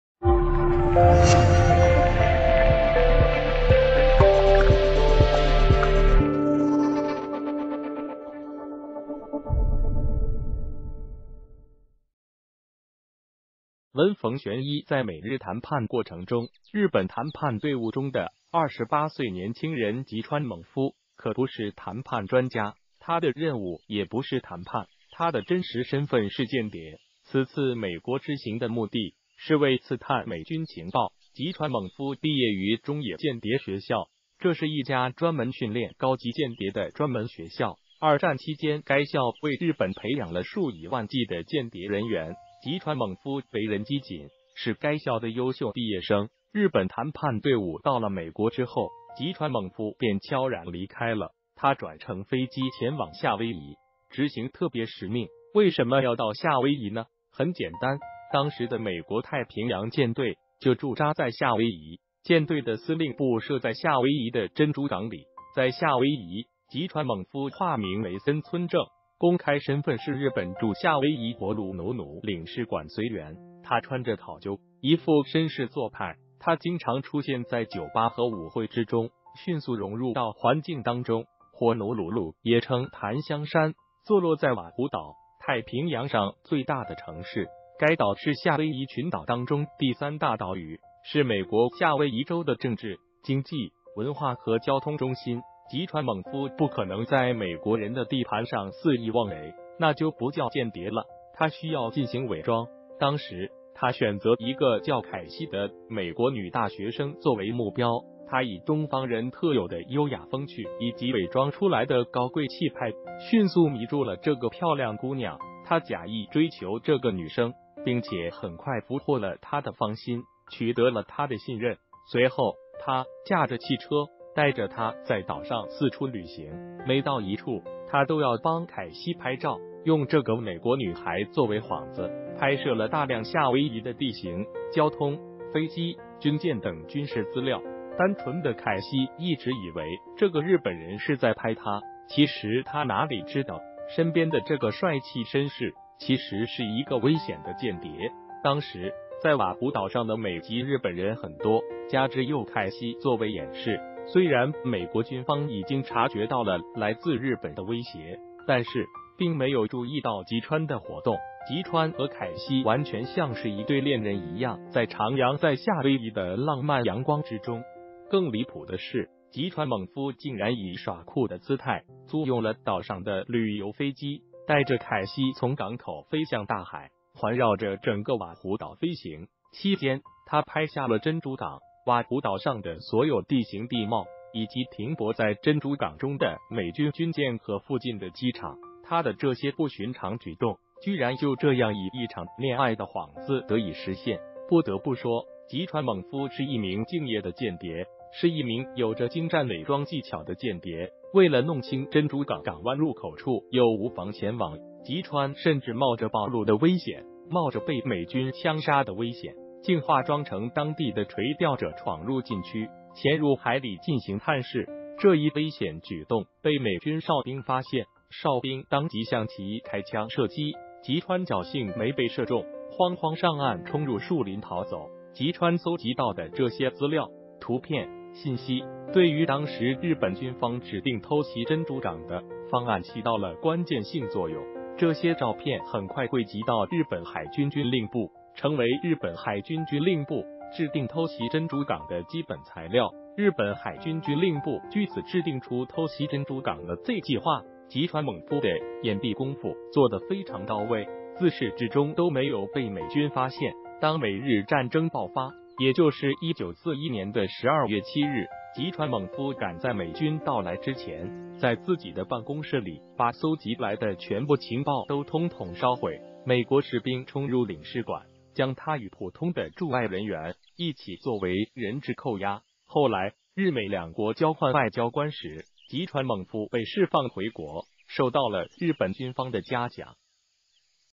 日本间谍勾引美国女子,以爱之名大肆刺探珍珠港军情
谢谢！日本间谍勾引美国女子,以爱之名大肆刺探珍珠港军情